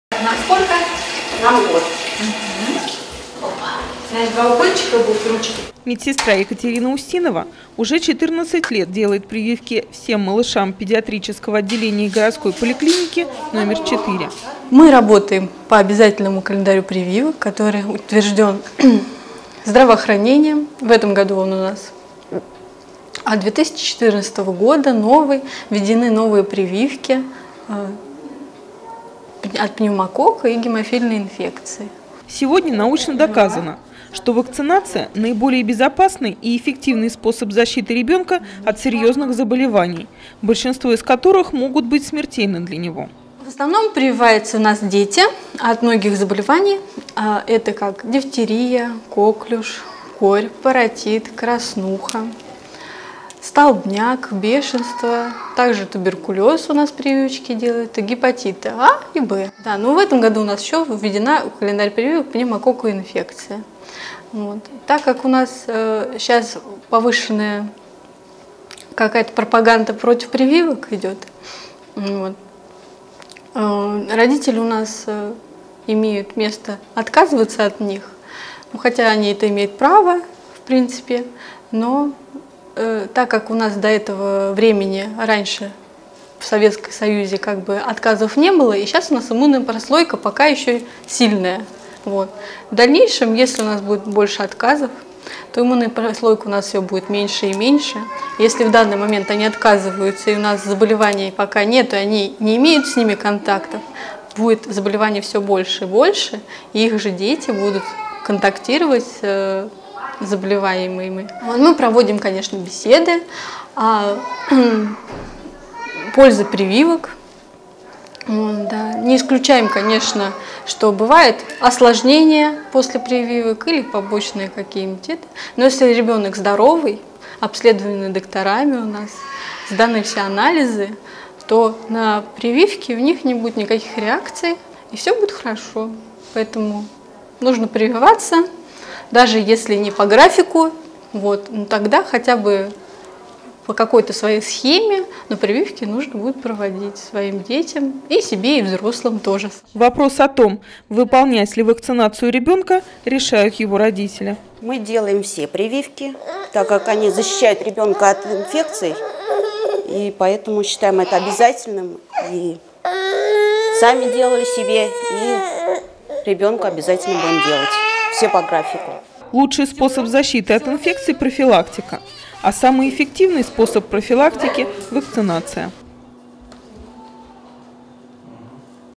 Прививки 07 05 15
С первой секунды после рождения человек подвергается влиянию огромного количества бактерий, в том числе болезнетворных. Чтобы укрепить иммунитет и защитить ребёнка от различных заболеваний в конце 17-го века английским хирургом Эдвардом Дженнером были изобретены препараты, которые стали основоположниками современной вакцинации. Какую роль играют прививки в нашей жизни смотрите в нашем сюжете.